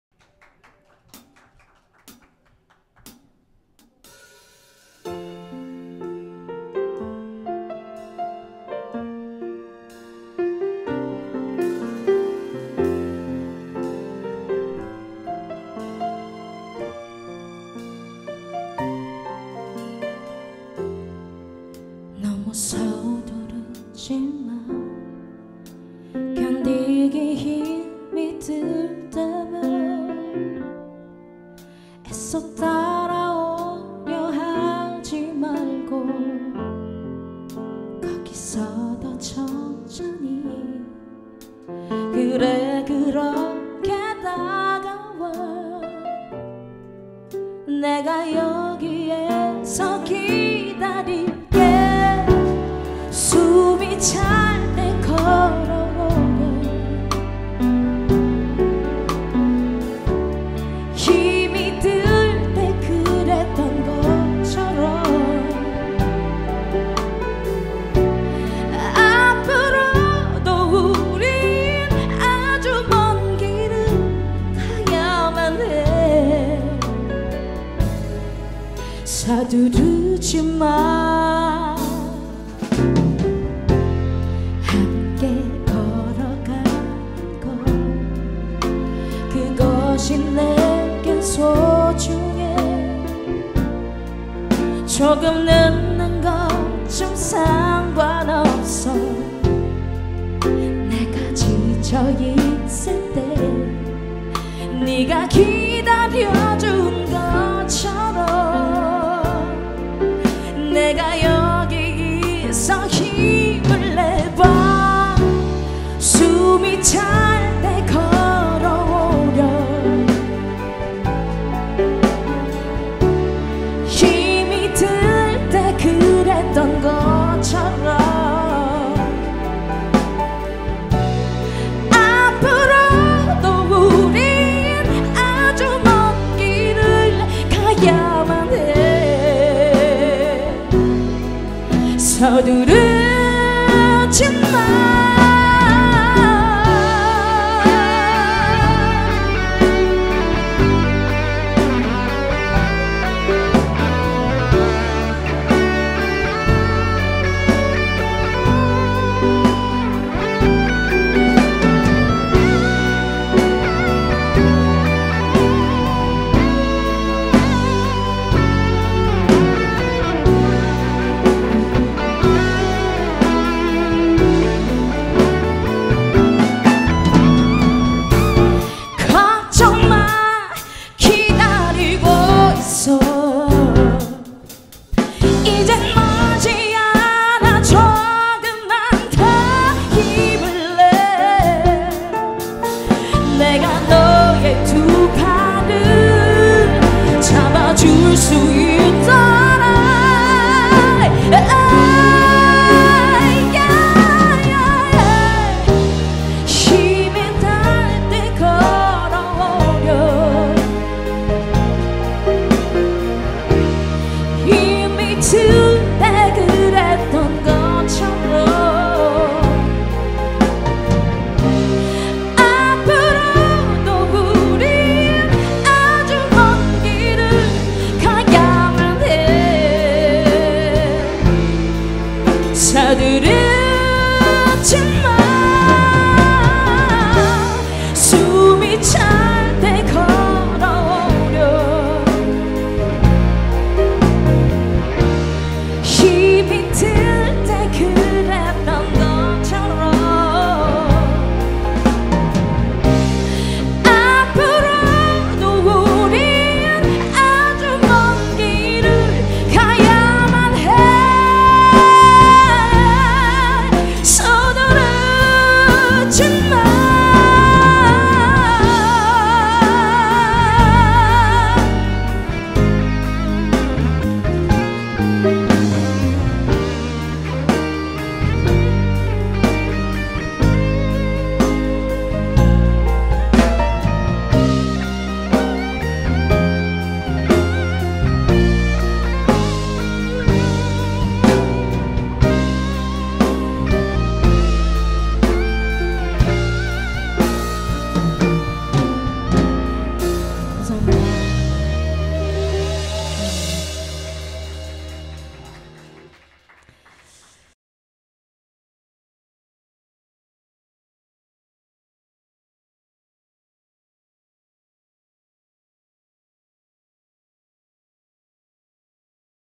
CLUB K SEOUL 2017/12/04 Soul Rain 공연_클럽케이서울
Vocal Soul Rain
Piano 김가형
Keyboard 박유진
Guitar 황시온
Bass 조성관
Drums 김영훈